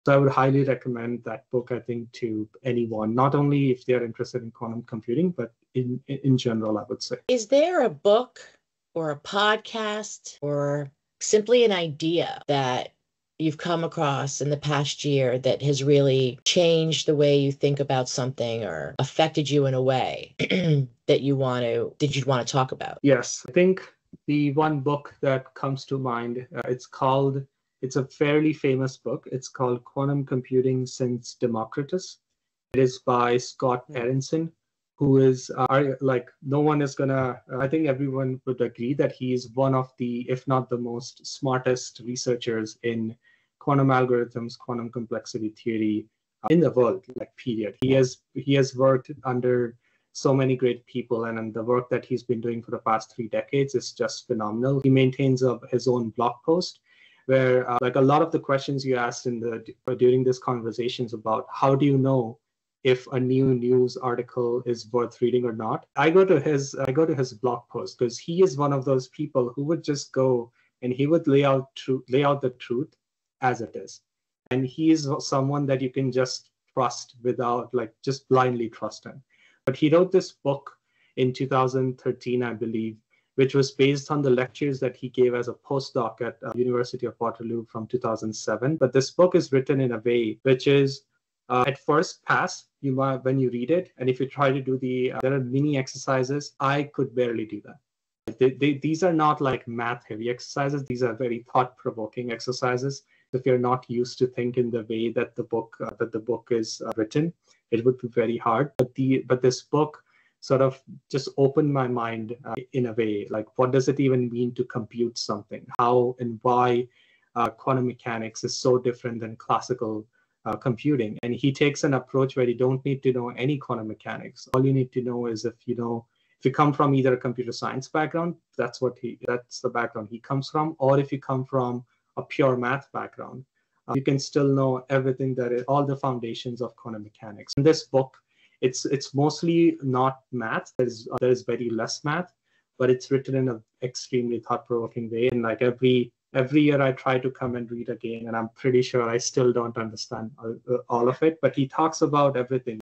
Quantum Computing: Mind-Blowing Insights from a Groundbreaking Book
Quantum Computing Since Democritus is highly recommended. This book approaches quantum mechanics from a computer science or math background. It explores what it means to compute something and why quantum mechanics differs from classical computing.